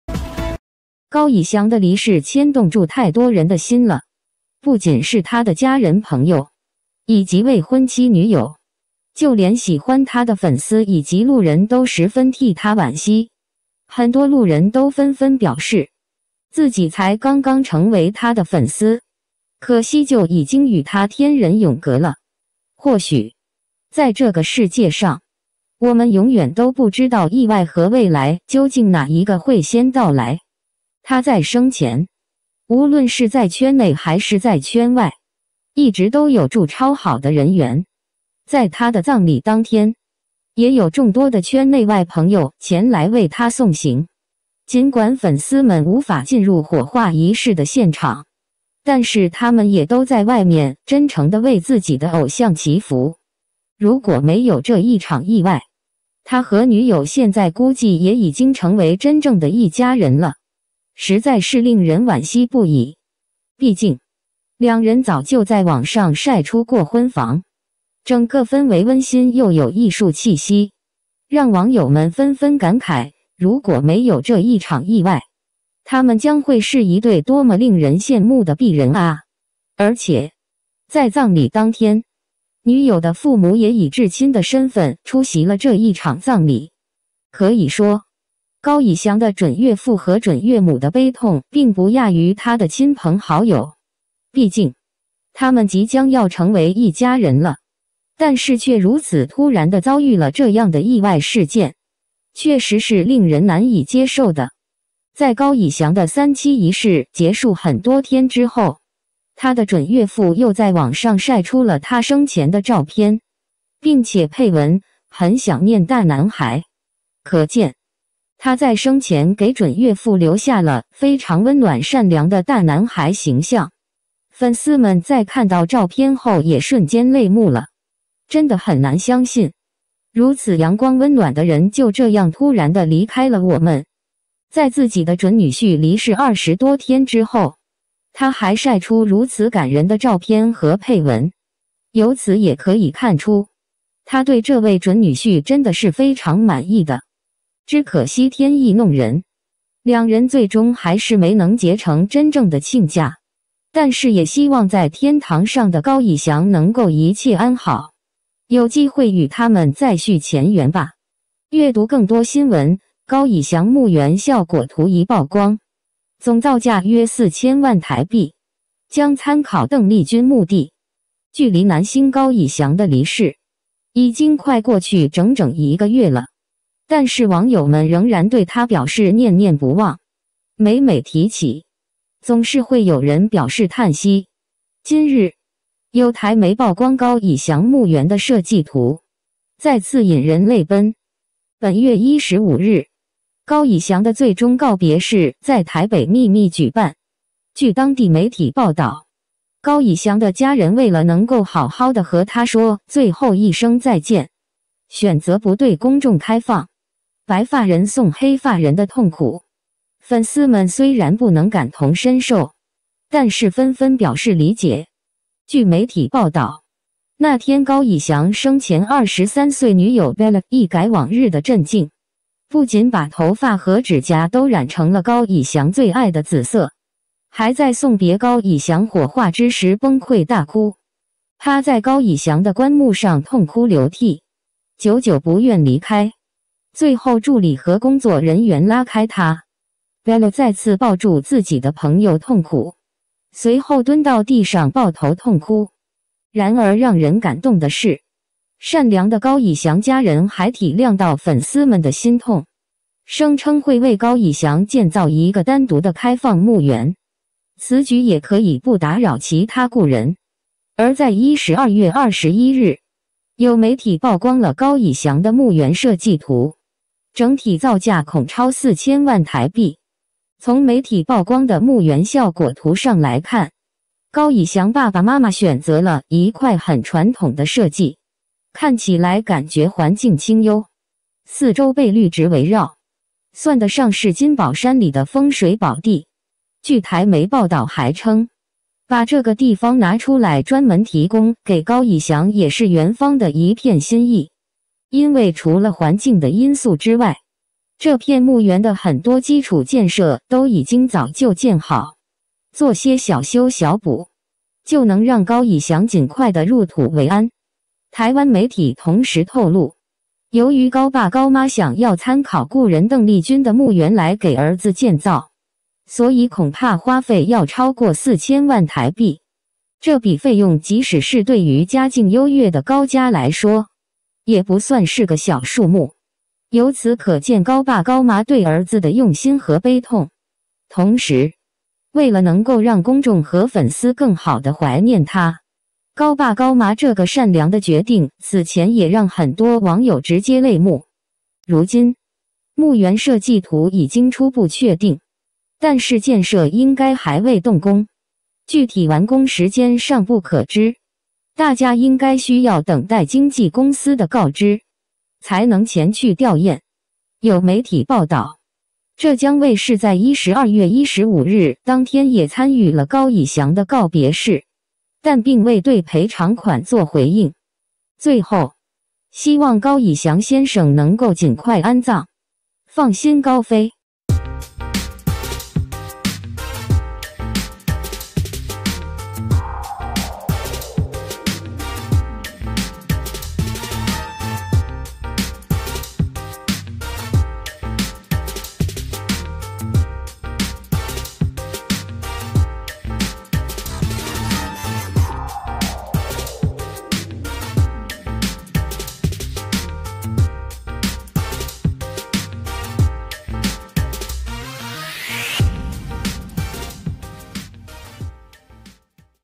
高以翔準岳父曬照發文，內容令人感慨萬千，生前曾深得準岳父喜愛
高以翔的離世牽動著太多人的心了，不僅是他的家人朋友，以及未婚妻女友，就連喜歡他的粉絲以及路人都十分替他惋惜，很多路人都紛紛表示：

自己才剛剛成為他的粉絲，可惜就已經與他天人永隔了，或許，在這個世界上，我們永遠都不知道意外和未來究竟哪一個會先到來。

他在生前，無論是在圈內還是在圈外，一直都有著超好的人緣，在他的葬禮當天，也有眾多的圈內外朋友前來為他送行，儘管粉絲們無法進入火化儀式的現場，但是他們也都在外面真誠地為自己的偶像祈福。

如果沒有這一場意外，他和女友現在估計也已經成為真正的一家人了，實在是令人惋惜不已。

畢竟，兩人早就在網上曬出過婚房，整個氛圍溫馨又有藝術氣息，讓網友們紛紛感慨：如果沒有這一場意外，他們將會是一對多麼令人羨慕的璧人啊，而且，在葬禮當天，女友的父母也以至親的身份出席了這一場葬禮。